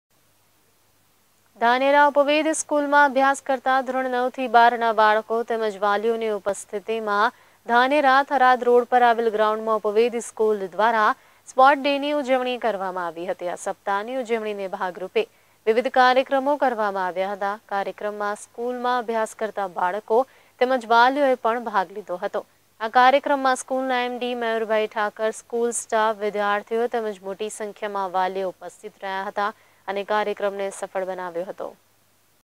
ધાનેરા : ઉપવેદ સ્કૂલ દ્વારા સ્પોર્ટ્સ ડેની ઉજવણી કરવામાં આવી
ધાનેરા ઉપવેદ સ્કૂલમાં અભ્યાસ કરતા ધોરણ ૯ થી ૧૨ ના બાળકો તેમજ વાલીઓની ઉપસ્થિતિમાં ધાનેરા થરાદ રોડ પર આવેલ ગ્રાઉન્ડમાં ઉપવેદ સ્કૂલ દ્વારા સ્પોર્ટ ડેની ઉજવણી કરવામાં આવી હતી. આ સપ્તાહની ઉજવણીના ભાગરૂપે વિવિધ કાર્યક્રમો કરવામાં આવ્યા હતા. આ કાર્યક્રમમાં સ્કૂલમાં અભ્યાસ કરતા બાળકો તેમજ વાલીઓએ પણ ભાગ લીધો હતો. આ કાર્યક્રમમાં સ્કૂલના એમડી મયુરભાઈ ઠાકર, સ્કૂલ સ્ટાફ, વિદ્યાર્થીઓ તેમજ મોટી સંખ્યામાં વાલીઓ ઉપસ્થિત રહ્યા હતા અને કાર્યક્રમને સફળ બનાવ્યો હતો